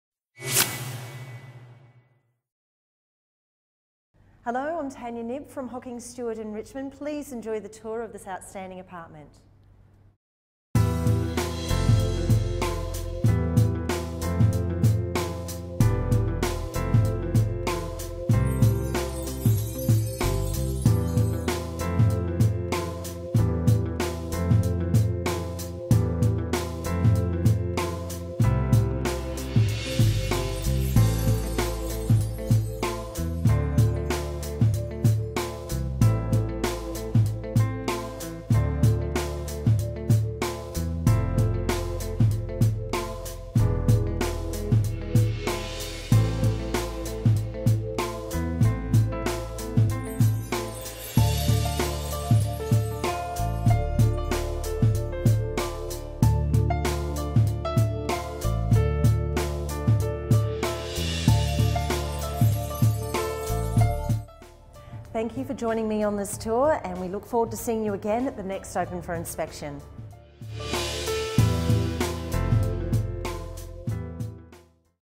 hockingstuart - Richmond - 208/633 Church Street - Tanya Knibb
208/633 Church Street, Richmond - With its fabulous outlook over the soon-to-be-completed Church Street Park, this generously proportioned apartment makes the perfect inner-city lifestyle / investment choice. Bathed in natural light, the apartment's versatile interior boasts two spacious bedrooms (master with access to terrace, 2nd bedroom with WIR) and dedicated study, stylish central bathroom, quality kitchen with stainless steel cooking appliances and dishwasher, exceptional open-plan living/dining area leading out to an extra-wide terrace with views over the park, Yarra River and beyond. Extras include heating and cooling, robes in both bedrooms, Euro laundry, secure entry, lift access and lower-level car space. Walk to Church Street / Chapel Street trams, shops and cafes, Yarra parks and trails.

Tanya Knibb : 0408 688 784 -